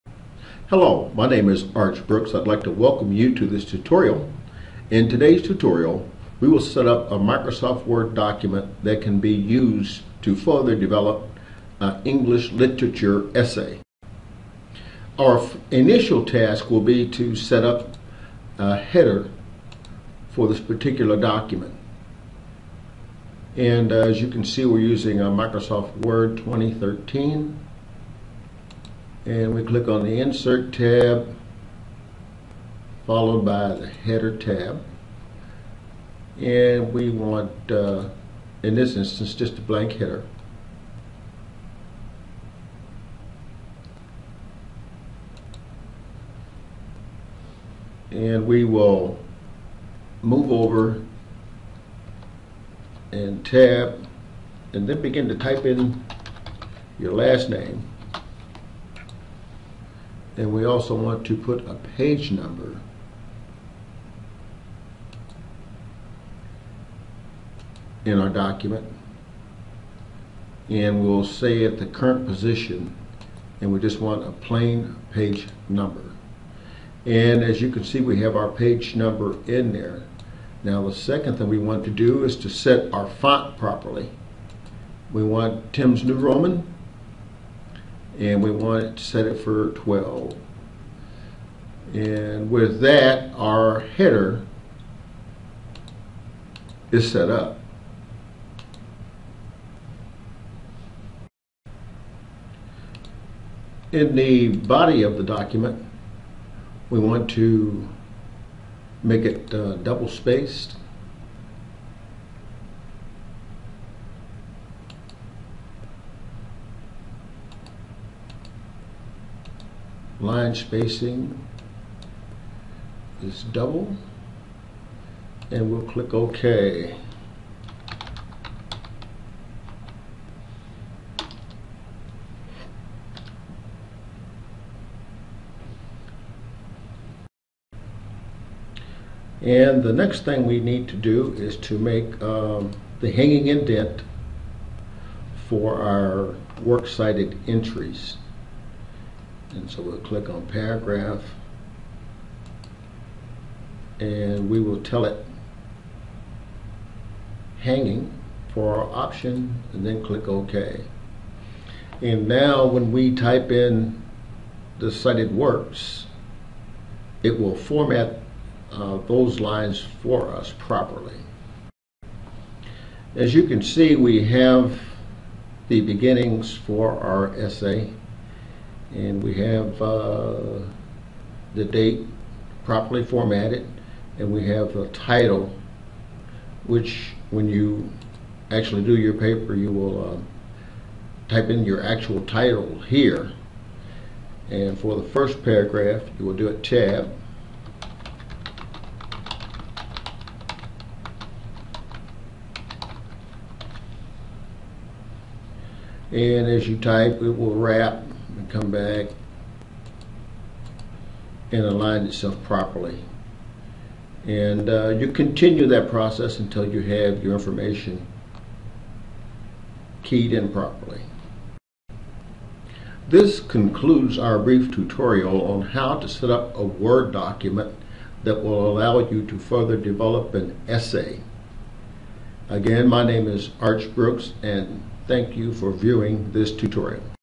Essay 01 Using Word Document
This video demonstrates how to set up Microsoft Word to enter and further develop an essay.